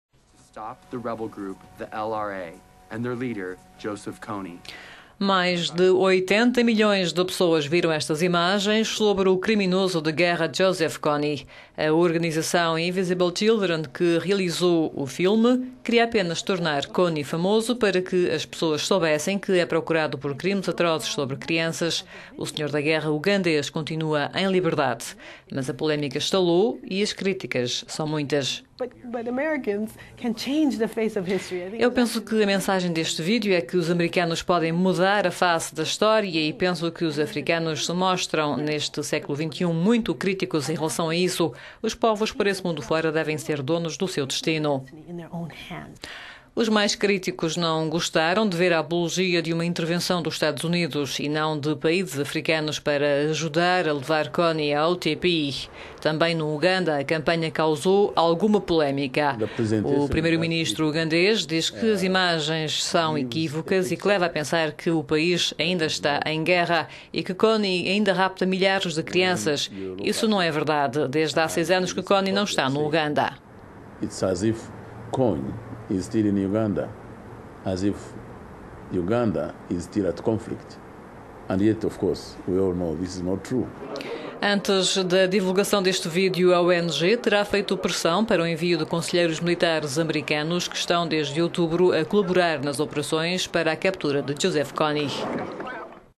Campanha kony envolta em polémica
Mais de 80 milhões de pessoas já viram estas imagens sobre o criminoso de guerra Joseph Kony.

Jason Russell, um dos fundadores da Organização Invisible Children, realizador do filme queria apenas "tornar Kony famoso", para que as pessoas soubessem é procurado por crimes  atrozes sobre criança mas o senhor da  guerra ugandês continua em liberdade. Mas polémica estalou e as críticas são muitas.

" Eu penso que a mensagem deste vídeo é que os americanos podem mudar a face da história e penso que os africanos se mostram muito críticos em relação a isso. Os povos por esse mundo fora devem ser donos do seu destino".

No Uganda a campanha da Invisible Children também causou polémica. O primeiro-ministro ugandês diz que as imagens são equívocas e leva a pensar que o país ainda está em guerra e que Kony ainda rapta  milhares de crianças. "Isto já não é verdade: desde há seis anos que Kony não está no Uganda"

Há ainda quem não tenha gostado ver a apologia de uma intervenção dos Estados Unidos - e não de países africanos - para ajudar a levar Kony ao TPI.

Antes da divulgação do vídeo a ONG terá feito pressão para o envio de conselheiros militares americanos que estão, desde Outubro passado, a colaborar nas operações de localização e captura de Kony.